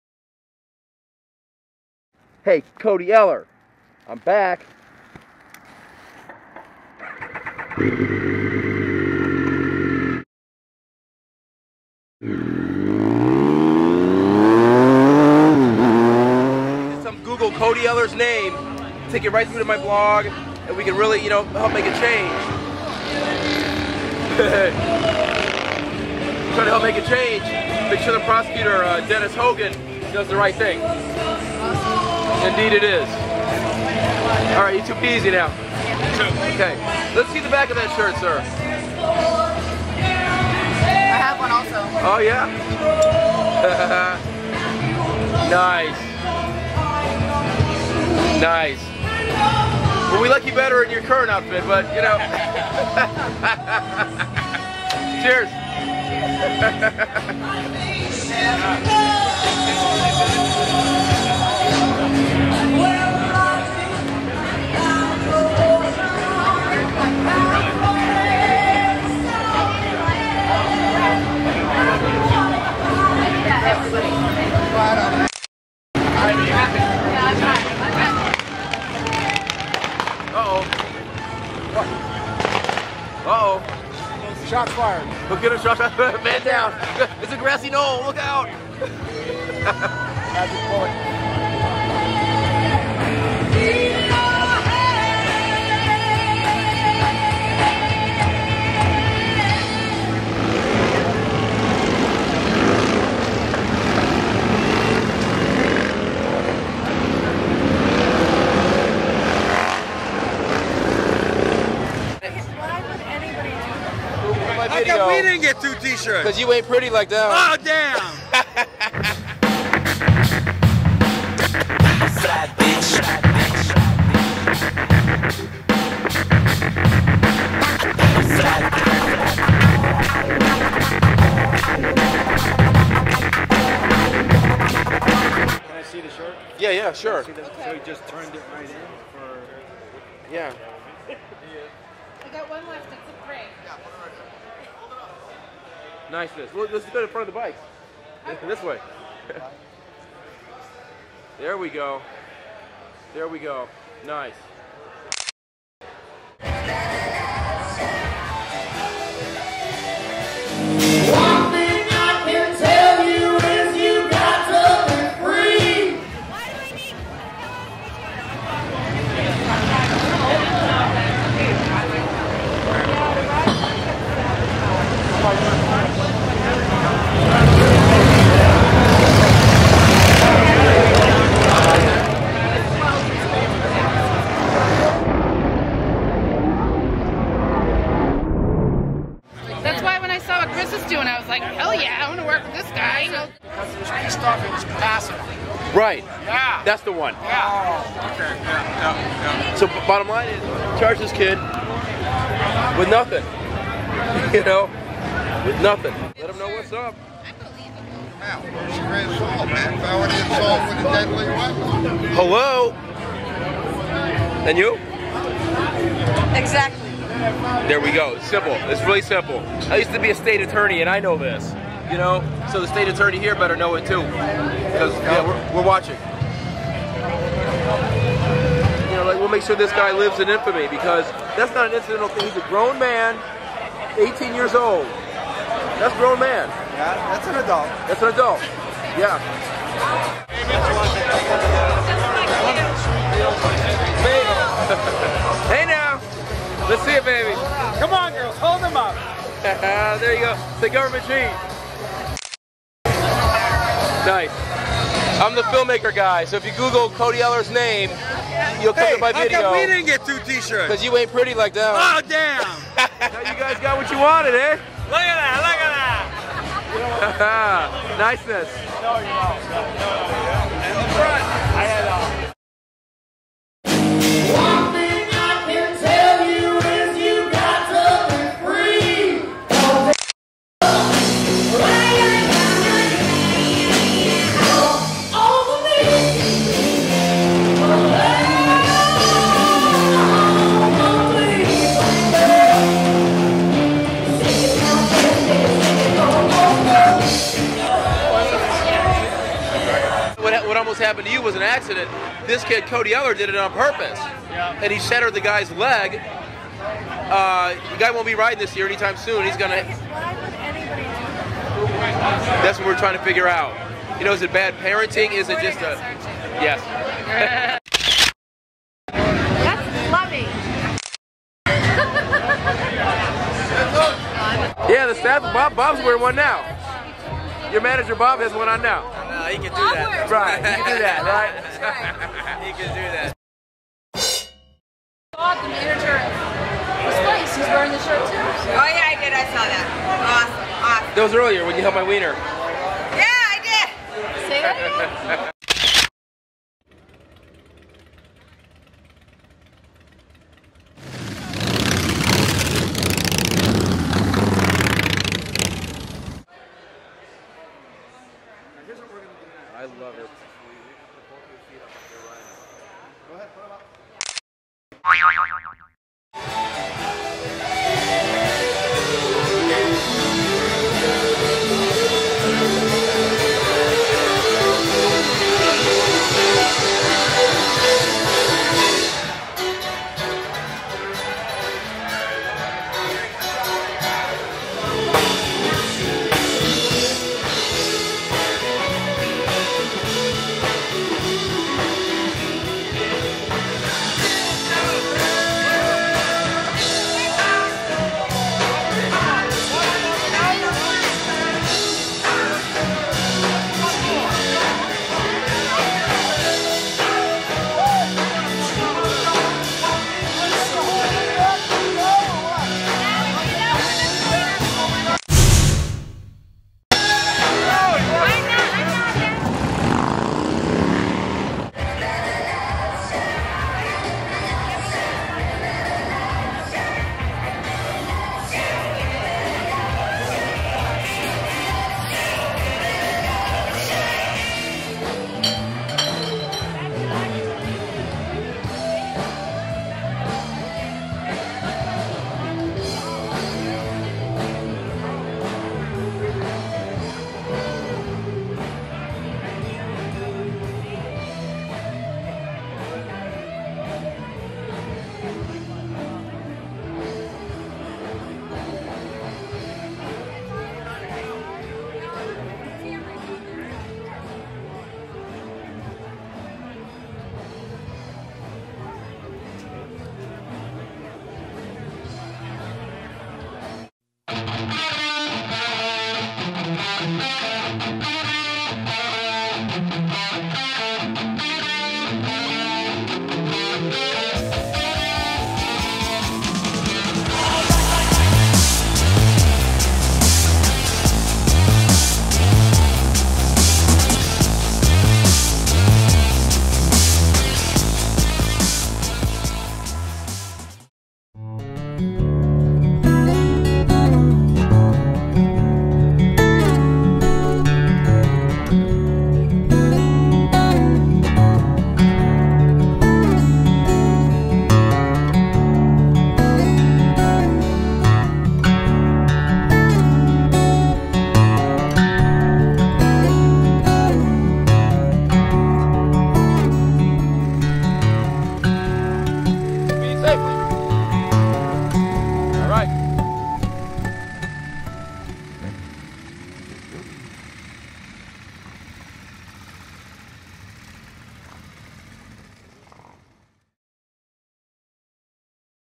KingCast Motorsports Bike Week 4th of July Cody Eller road rage movie
Trial Date 20 August 2012

Repeat after me when you talk to the Grand Jury Attorney Hogan:

Assault with an automobile is inherently Assault with a Deadly Weapon and is therefore First Degree Assault, ipso facto. So don't get it twisted as you did with that certain Divorce case. Anyway the movie is exporting and I will upload it by morning. It's awesome, I am very proud of it and the message that is conveyed. Bikers need to stand up and stop taking the bullshit, here is the Bike Week journal entry#1 image gallery with links to the first movie and much, much more. Peace.